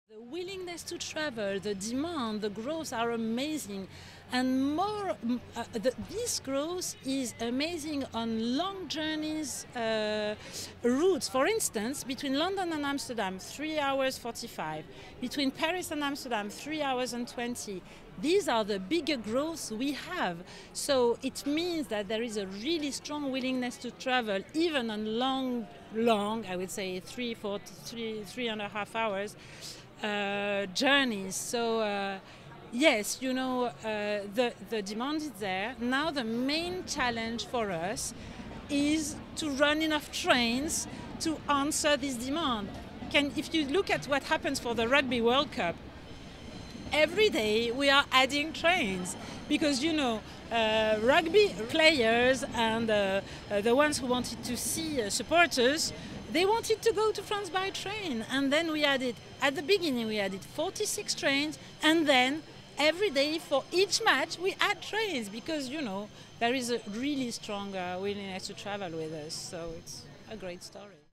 Eurostar boss says there is a 'strong willingness' to travel by train
Eurostar’s chief executive says Kent’s two currently disused stations on the high-speed line to Paris, Brussels and Amsterdam will only reopen “once we can afford it”.

Speaking at the brand launch for Eurostar’s merger with rail firm Thalys, Gwendoline Cazenave said her priority was on ensuring the passenger experience at London and Paris is the “best it can be”.

Ebbsfleet International and Ashford International were part of the original High Speed 1 plan connecting London St Pancras International with the Channel Tunnel.